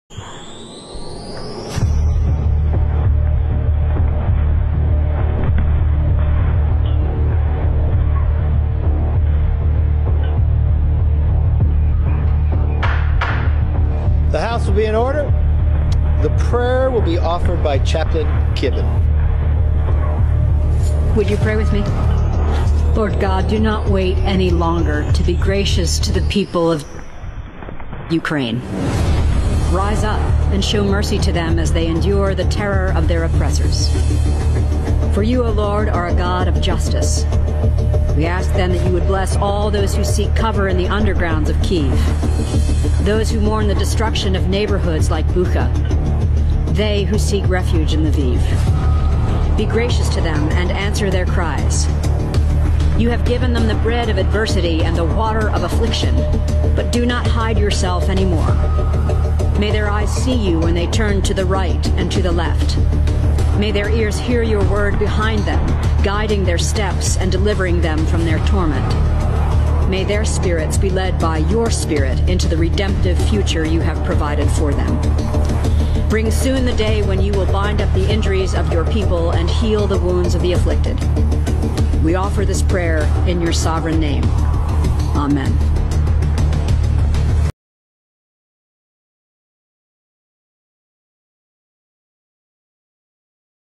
The House of Representatives! with a unique prayer!
@FoxNews Government War USA Ukraine, UkraineWar Ukraine,Russia.

House of representatives opened up with an unusual prayer. There is a prayer that they must include others. However they didn't. If you Google it you'll see the way they're supposed to pray. All I can say is bravo thank you Jesus love you God.